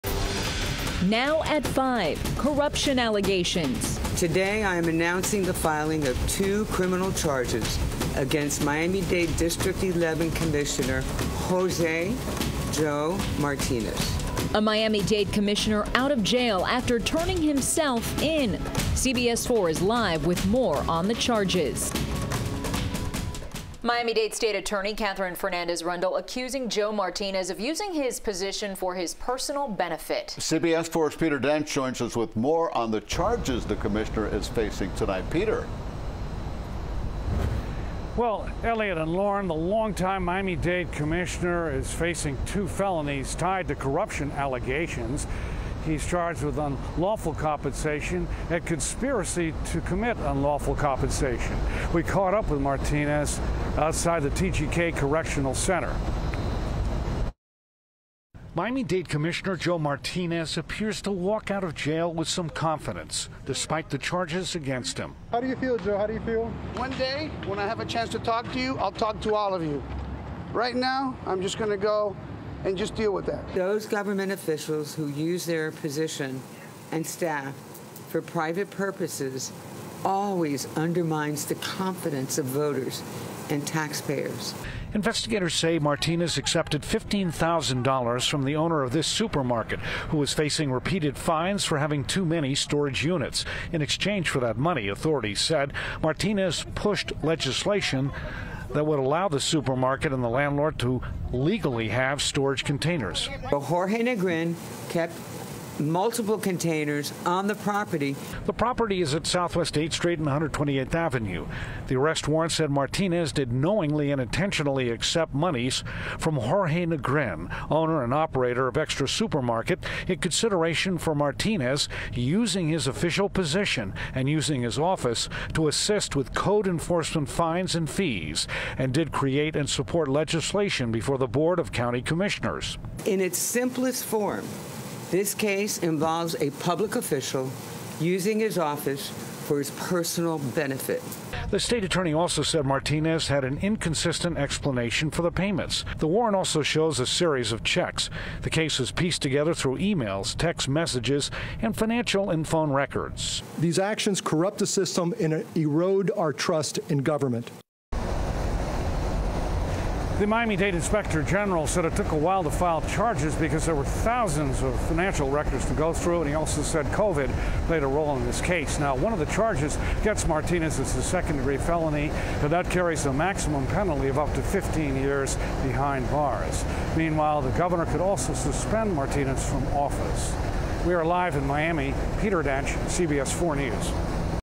Miami-Dade Commissioner Out Of Jail After Corruption Charges Are Filed
Miami-Dade County District 11 Commissioner Joe Martinez faces two criminal charges stemming from a public corruption investigation.